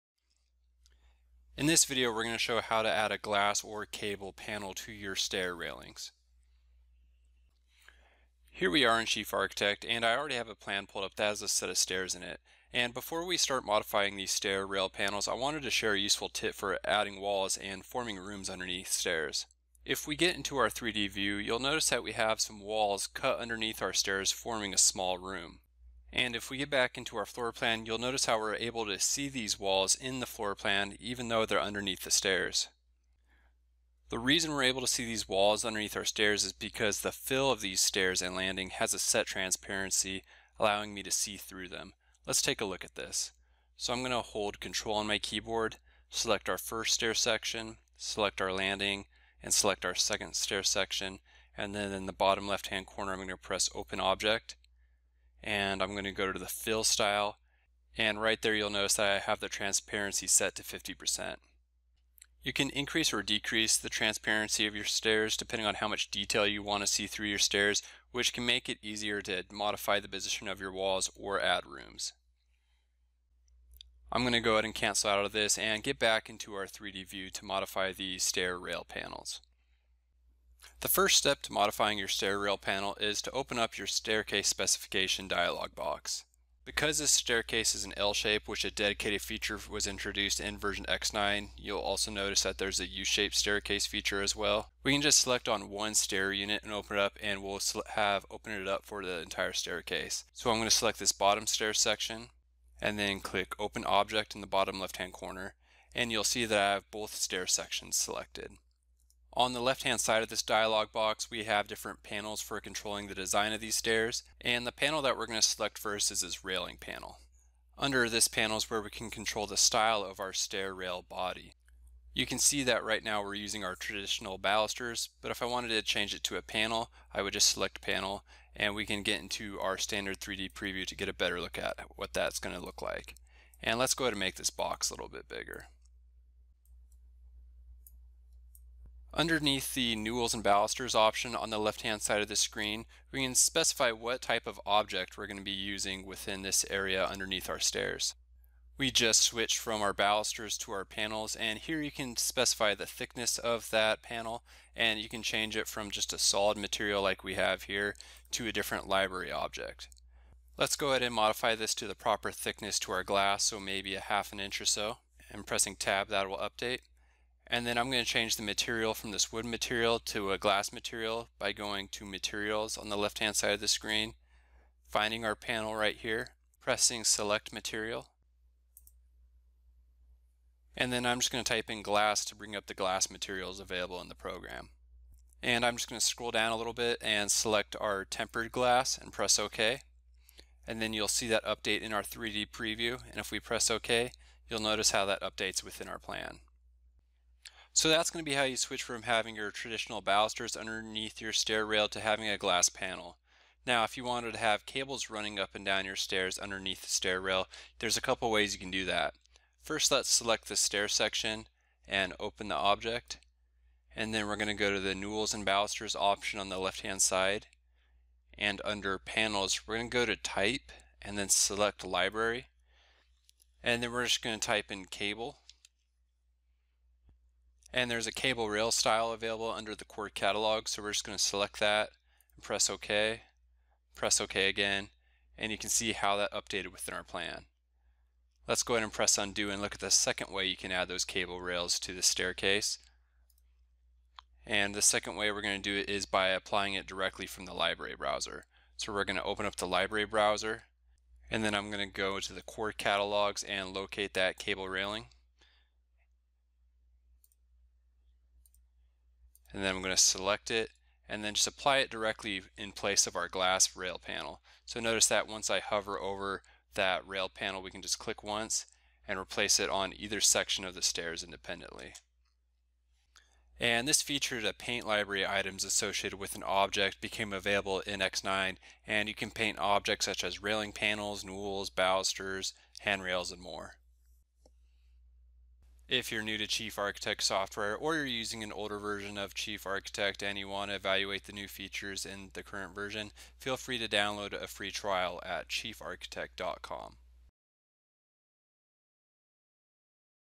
Adding Glass or Cable Rail Panels to Stairs
Learn how to apply glass or cable panels to your staircase using the Stair Specification dialog or using the Replace From Library function.